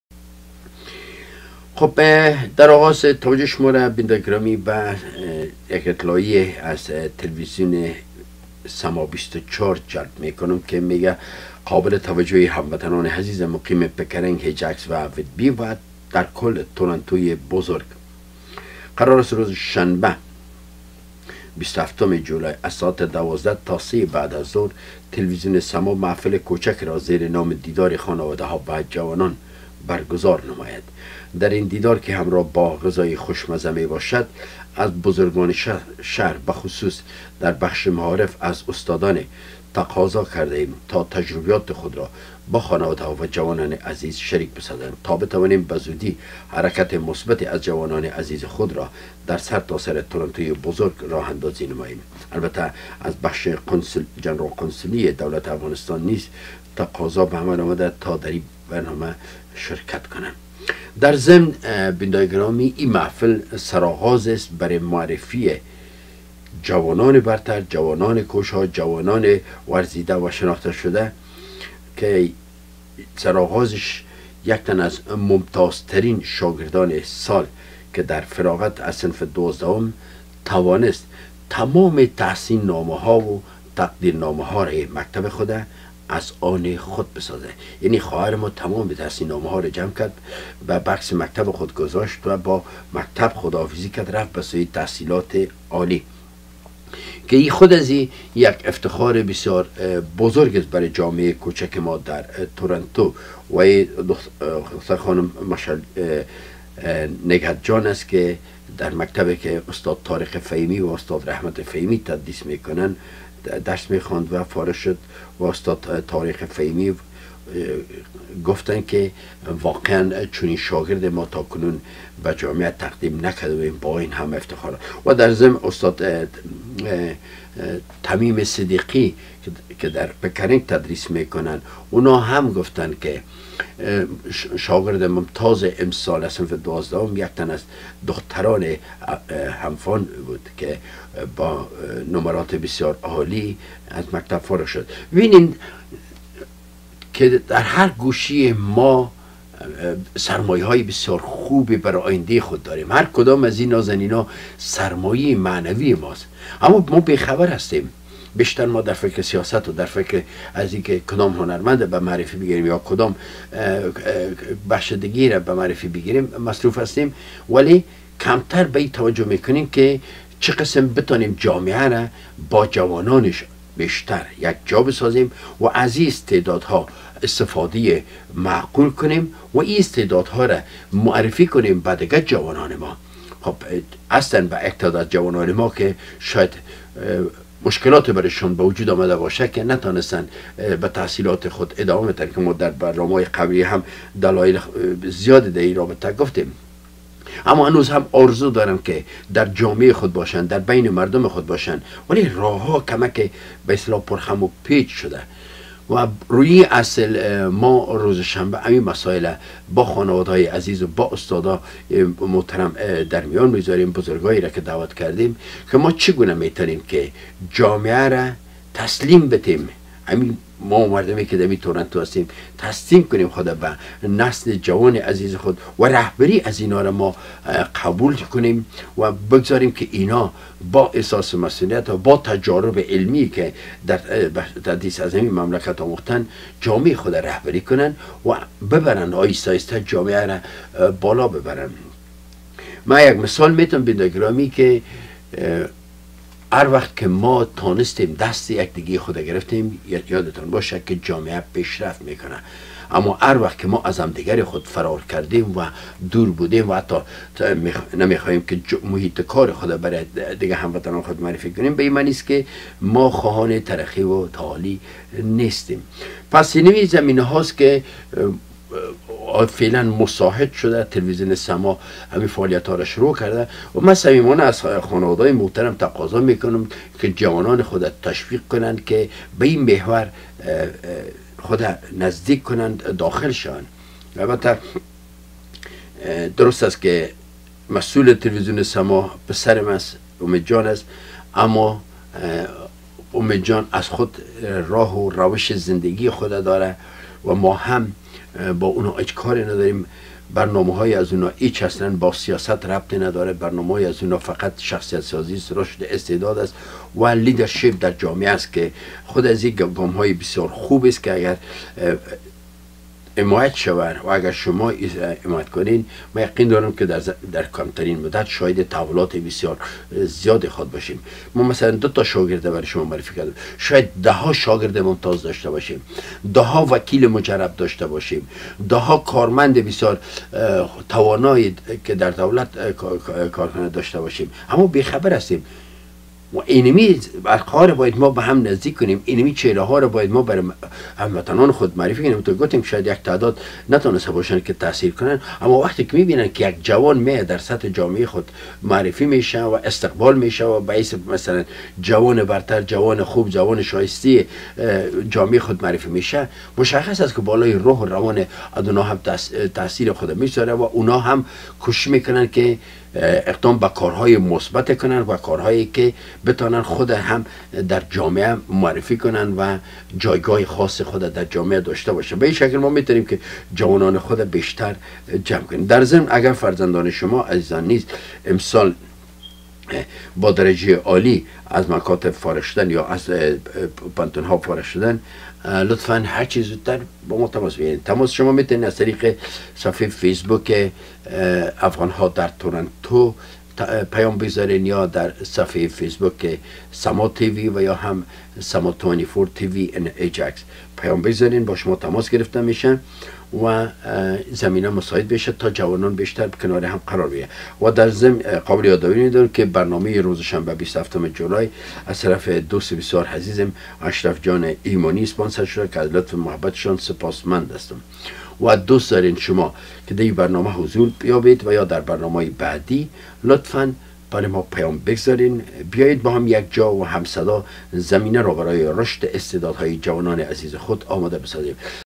برنامه ی: برای رشد استعداد های جوانان در تورنتو
تلویزیون سما24 تصمیم دارد بعد از این از جوانان موفق و برتر شهر تورنتو نه تنها قدردانی نماید بلکه انها به معرفی نیز میگیرد تا سرمشقی باشد برای دیگر جوانان عزیز ما